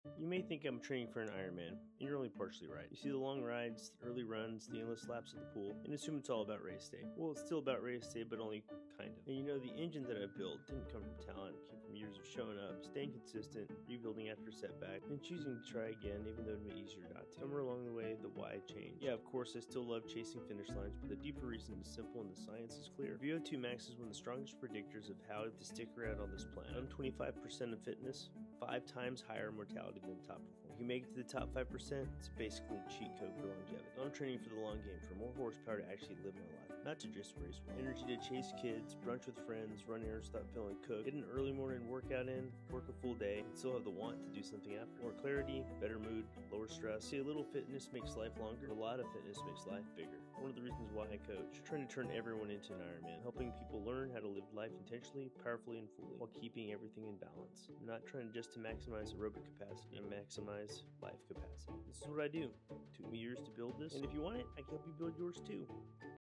I’m not just training for an Ironman
My VO₂max: 57 at age 45.
That puts me near the top 1-5% of fitness for my age — and the science on what that means is wild:

📊 Mortality & Fitness — The Hard Numbers
• Individuals in the lowest fitness quartile have a 4.5–5× higher all-cause mortality rate than those in the elite fitness group.
• Even moving from “low” to “below average” fitness drops mortality risk by 50%.
• Increasing fitness by 1 MET (~3.5 VO₂ points) reduces all-cause mortality by 10–15%, regardless of age, sex, or baseline health.
• High cardiorespiratory fitness reduces risk of:
• Cardiac mortality by ~60%
• Stroke by ~40%
• Cancer mortality by 20–30%
• Death from metabolic disease (T2D & metabolic syndrome) by 50%+

In medicine, we don’t see many levers this powerful.

Cardiorespiratory fitness is basically a biological insurance policy.

This is why Ironman is a lifestyle for me — not for medals (although they are cool ngl), but because fitness is one of the most potent predictors of living longer and living better.

If you’re interested in talking about how i can help you shoot me a dm. K bye.